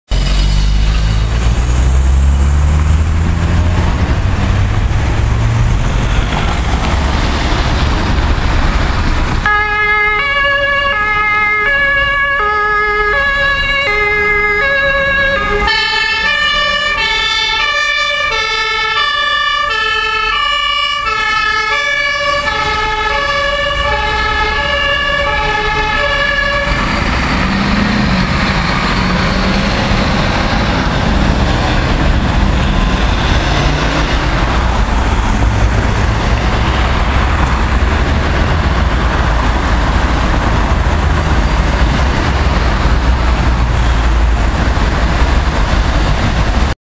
Kurzzug BF Hamburg F15 Stellingen
KLF+HLF1(MAN F05)+DLK rücken mit Pressluft aus!